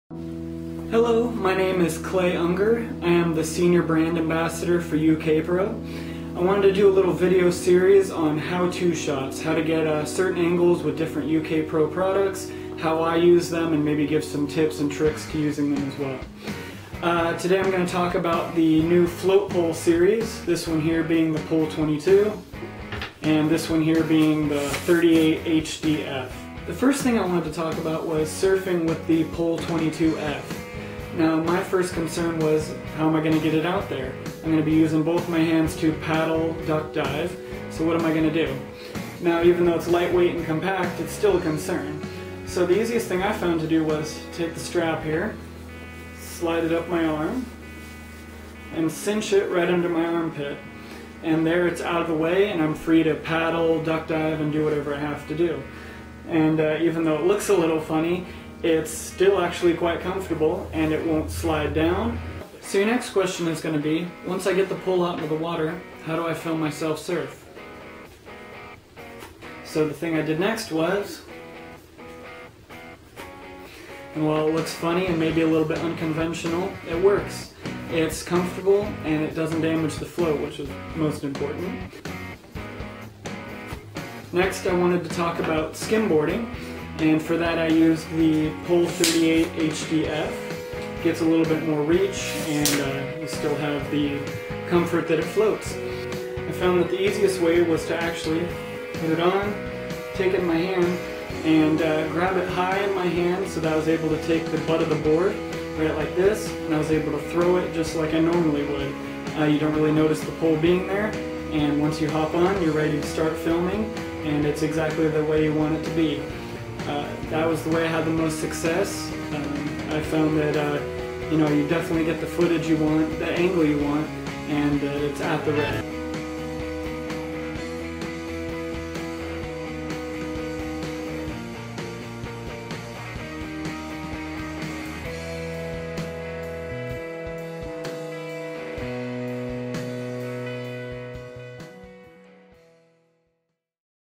Street Team Spotlight - Clay's Tips on UKPro Float Poles and How to Film in the Surf
UKPro Brand Ambassador Clay Unger (@clayunger) talks about the UKPro float poles and how to get the best out of them. Whether surfing or skimboarding, Clay has some tips on how to get out there and get the coolest shots while keeping your camera out of the way and safe in your hands.